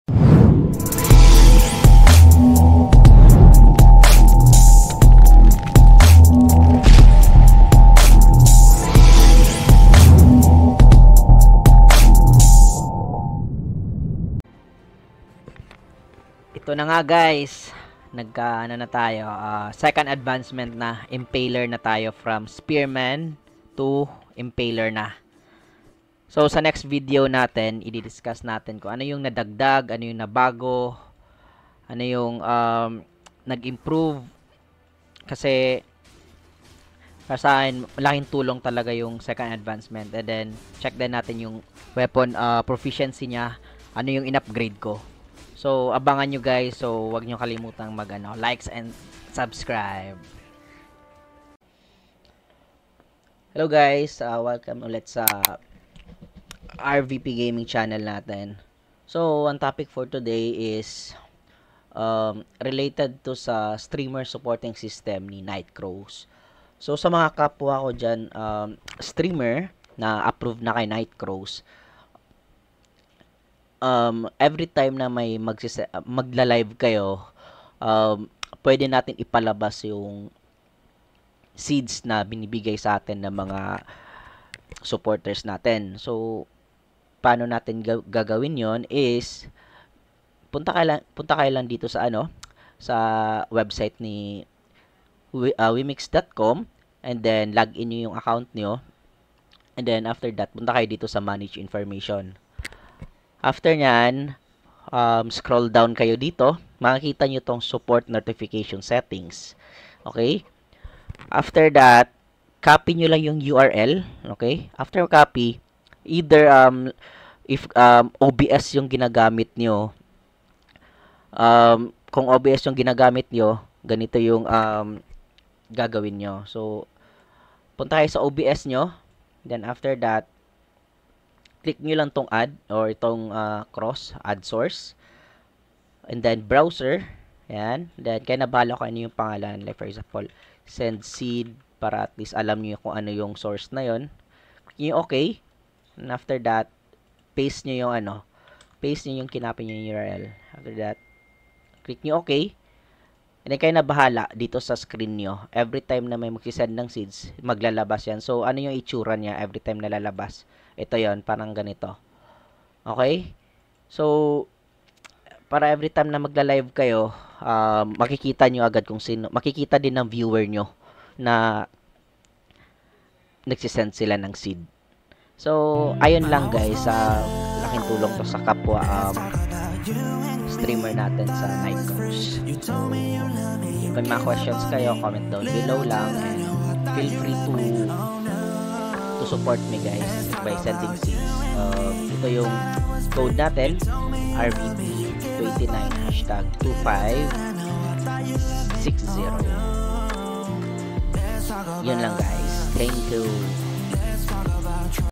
HOW TO SET-UP STREAMER SUPPORT NOTIFICATION SETTINGS ON NIGHT CROWS GLOBAL
Hi guys! On this video, I  will guide you on how to set-up your streamer support notification settings on nights crows global if you are an approved night crows global streamer.

Timestamps:
00:00 RvP Gaming intro
0:16  streamer support introduction
0:53 how to find streamer notification link
1:32 how to add streamer notification link on OBS
2:17 outro